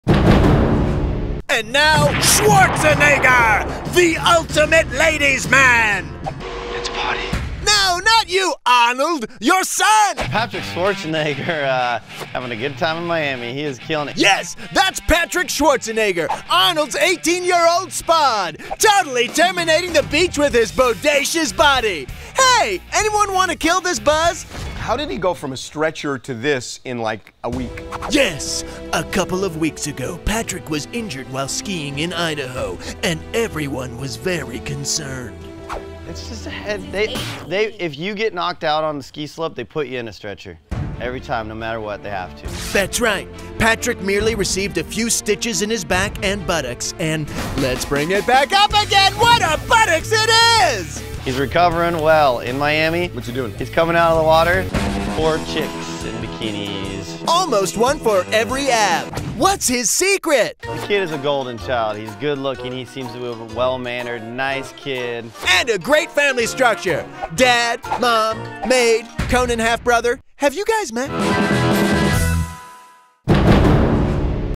Patrick Schwarzenegger -- Like Father, Like Son | TMZ
As if you needed any more evidence that Patrick Schwarzenegger's life is AWESOME -- check out the dude rolling around with four HOT bikini babes on the beach. Arnold must be proud.